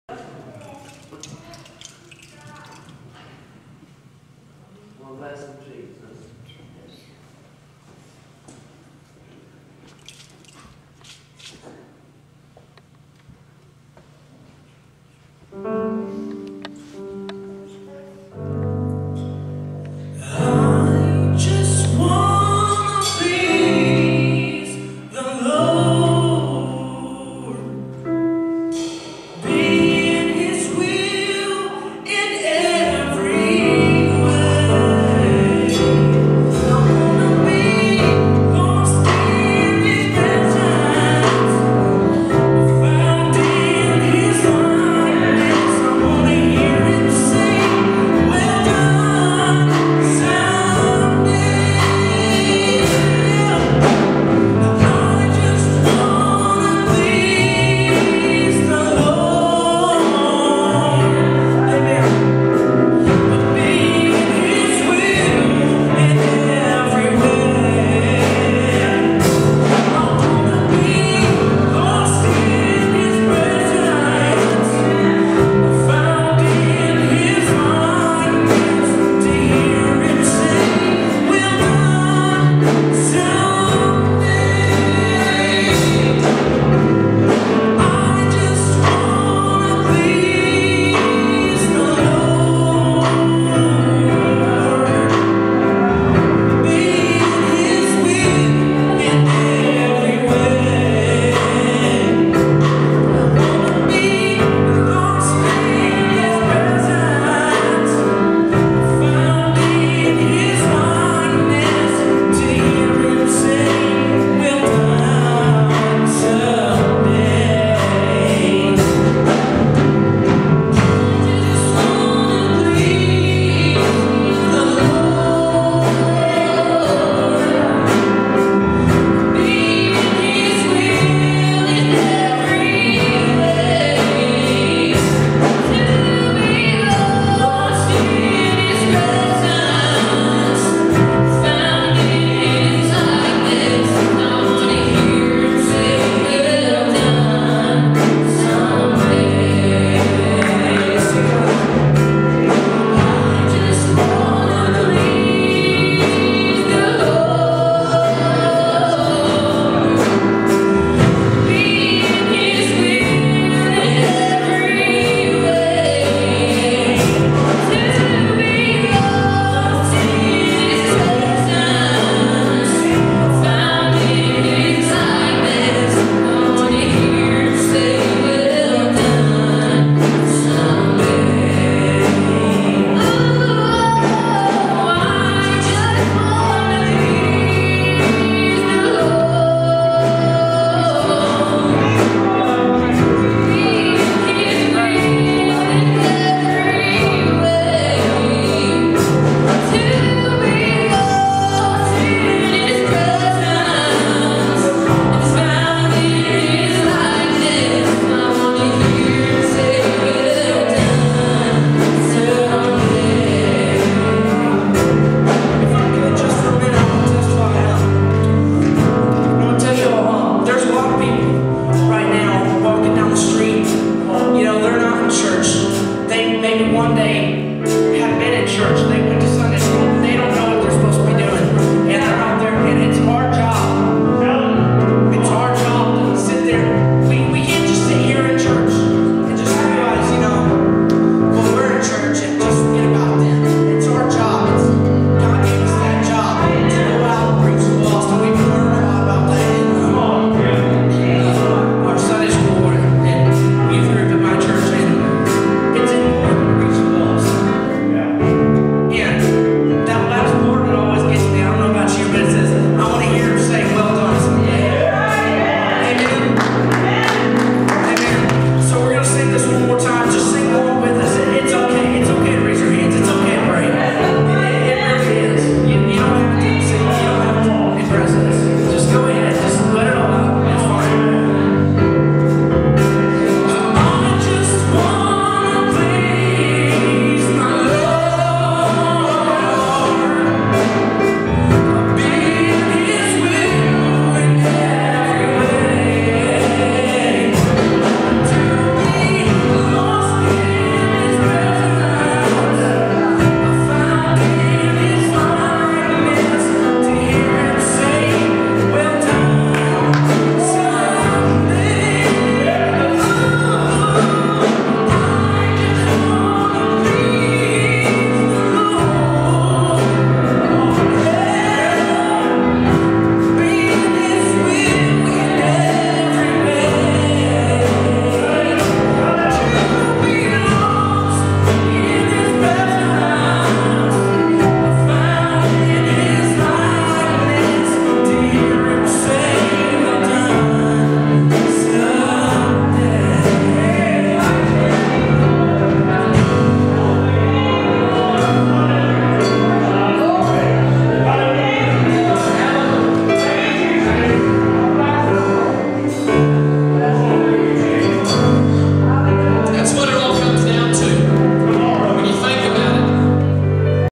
Crossroad Pentecostal Church of God - Bro Isaac Dement and Sis Hannah Herrera Singing
Youth Group Singing